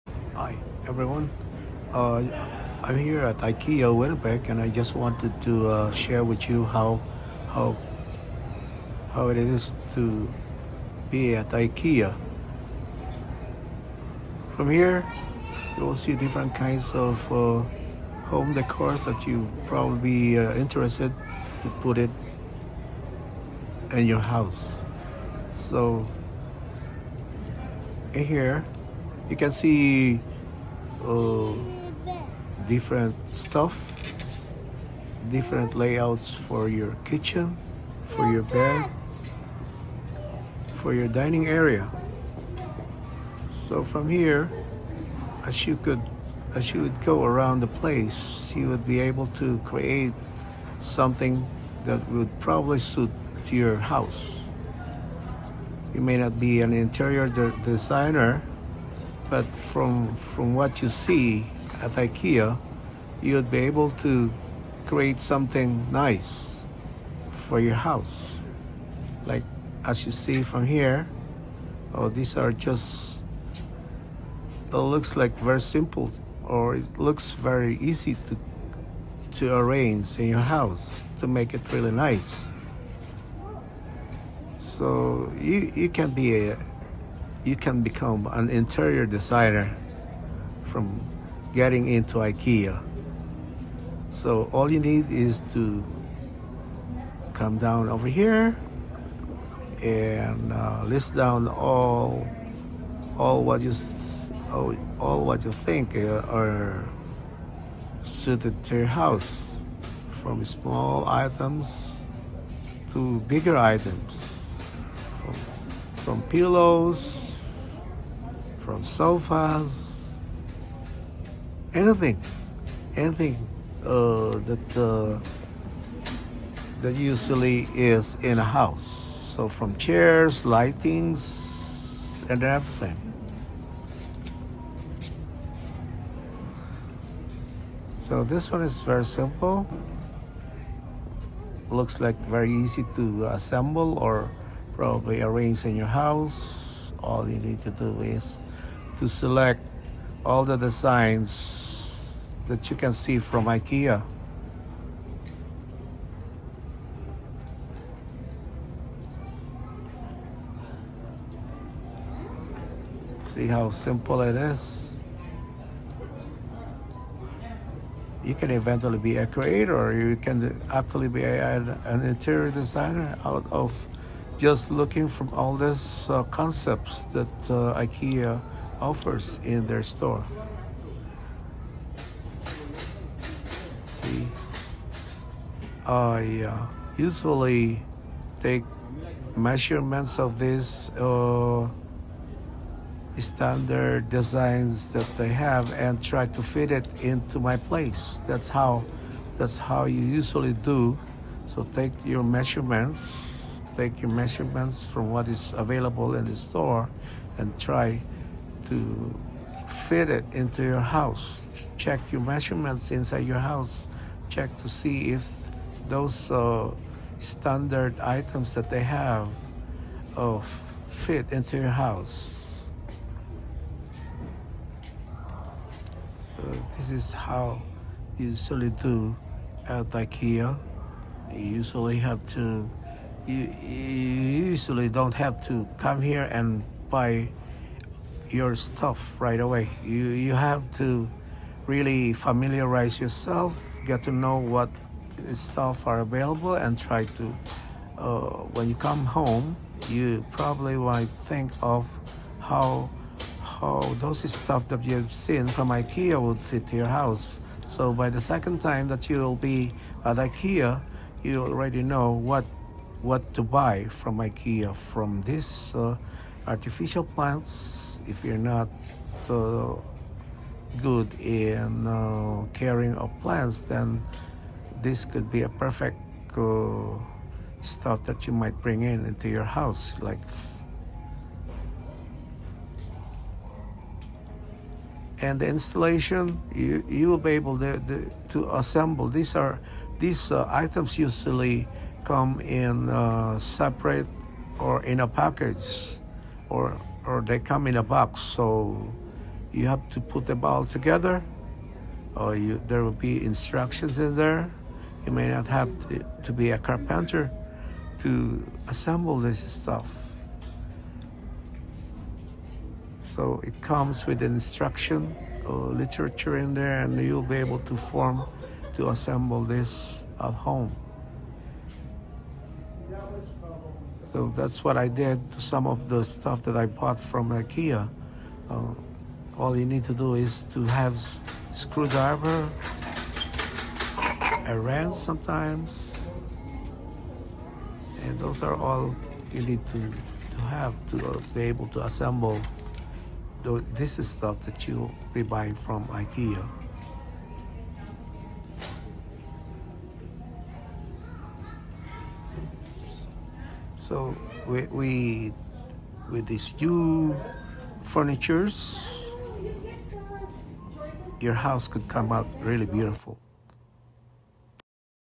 How to be a Home Interior Designer @ IKEA
Visiting IKEA is not just a wonderful experience but could arouse your creativeness in home decorations. A how to experience out of the many design concepts that you see at IKEA.